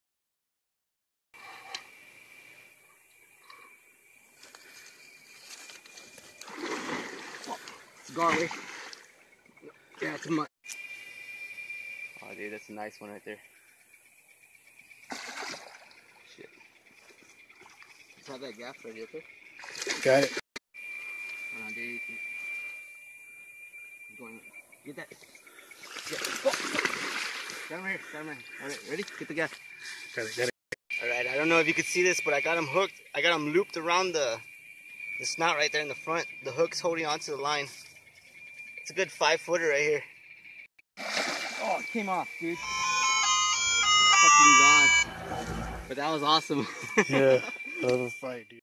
lost the fish but far from sad
the hook looped around its mouth ive seen that before but my first time having that happen while fishing